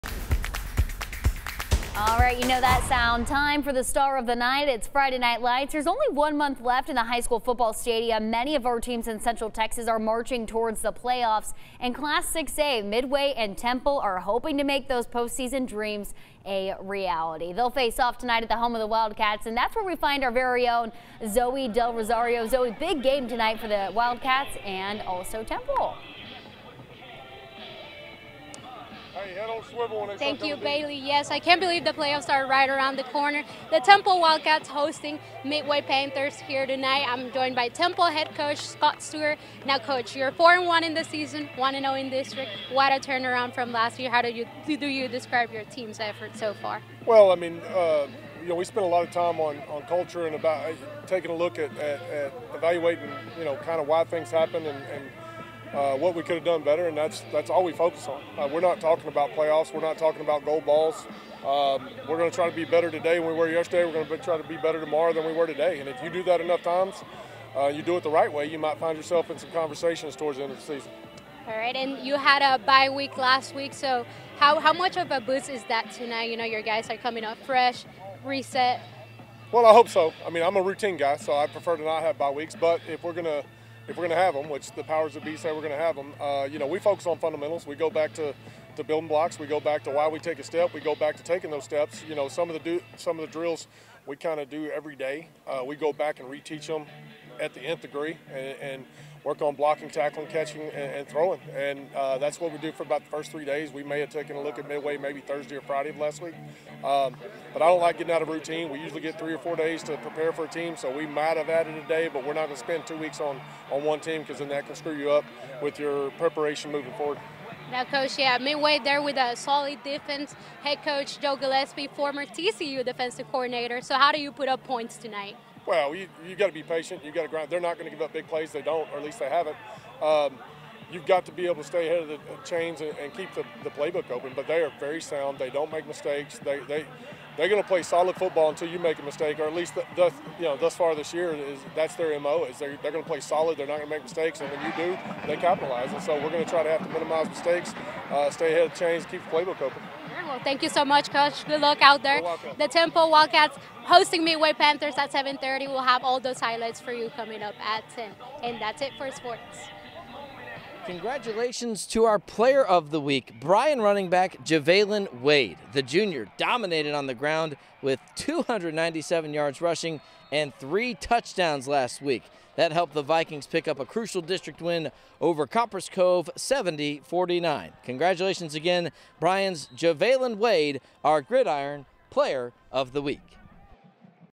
Friday Night Lights - Midway vs Temple
Zoe Del Rosario at Wildcat Stadium talking to Temple Head Coach Scott Stewart.
Read more on KCENTV.COM